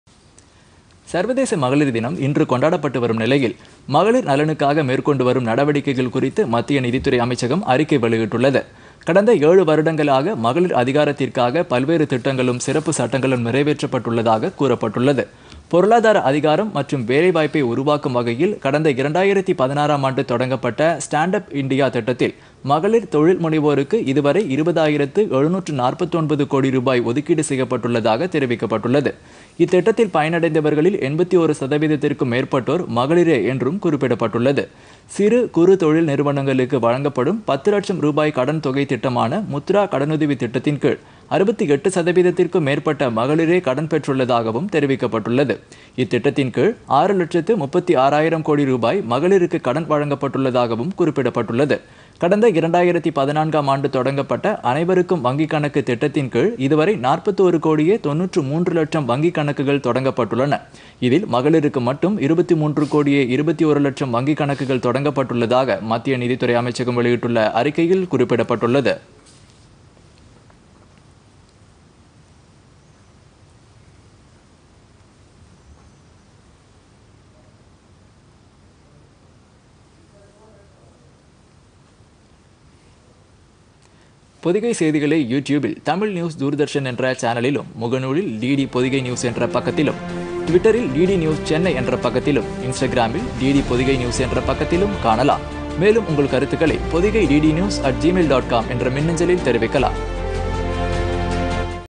பல்வேறு திட்டங்களில் மகளிர் பயன் - நிதியமைச்சகம்
சர்வதேச மகளிர் தினம் இன்று கொண்டாடப்பட்டு வரும் நிலையில், மகளிர் நலனுக்காக மேற்கொண்டு வரும் நடவடிக்கைகள் குறித்து  மத்திய நிதித்துறை அமைச்சகம் அறிக்கை வெளியிட்டுள்ளது.
   கடந்த ஏழு வருடங்களாக, மகளிர் அதிகாரத்திற்காக பல்வேறு திட்டங்களும், சிறப்பு சட்டங்களும் நிறைவேற்றப்பட்டுள்ளதாகக் கூறப்பட்டுள்ளது.
   பொருளாதார அதிகாரம் மற்றும் வேலை வாய்ப்பை உருவாக்கும் வகையில், கடந்த 2016-ம் ஆண்டு தொடங்கப்பட்ட, Stand up India  திட்டத்தில், மகளிர் தொழில் முனைவோருக்கு, இதுவரை 20 ஆயிரத்து 749 கோடி ரூபாய் ஒதுக்கீடு செய்யப்பட்டுள்ளதாகத் தெரிவிக்கப்பட்டுள்ளது.
   இத்திட்டத்தில் பயனடைந்தவர்களில் 81 சதவீதத்திற்கும் மேற்பட்டோர், மகளிரே என்றும் குறிப்பிடப்பட்டுள்ளது.
   சிறு, குறு தொழில் நிறுவனங்களுக்கு வழங்கப்படும் பத்து லட்சம் ரூபாய் கடன் தொகை திட்டமான, முத்ரா கடனுதவி திட்டத்தின் கீழ், 68 சதவீதத்திற்கும் மேற்பட்ட மகளிரே கடன் பெற்றுள்ளதாகவும் தெரிவிக்கப்பட்டுள்ளது.
  இத்திட்டத்தின் கீழ்,  ஆறு லட்சத்து 36 ஆயிரம் கோடி ரூபாய், மகளிருக்கு கடன் வழங்கப்பட்டுள்ளதாகவும் குறிப்பிடப்பட்டுள்ளது.
    கடந்த 2014-ம் ஆண்டு தொடங்கப்பட்ட, அனைவருக்கும் வங்கிக் கணக்கு திட்டத்தின் கீழ்,  இதுவரை 41 கோடியே 93 லட்சம் வங்கிக் கணக்குகள் தொடங்கப்பட்டுள்ளன.
  இதில் மகளிருக்கு மட்டும், 23 கோடியே 21 லட்சம் வங்கிக் கணக்குகள் தொடங்கப்பட்டுள்ளதாக, மத்திய நிதித்துறை அமைச்சகம் வெளியிட்டுள்ள அறிக்கையில் குறிப்பிடப்பட்டுள்ளது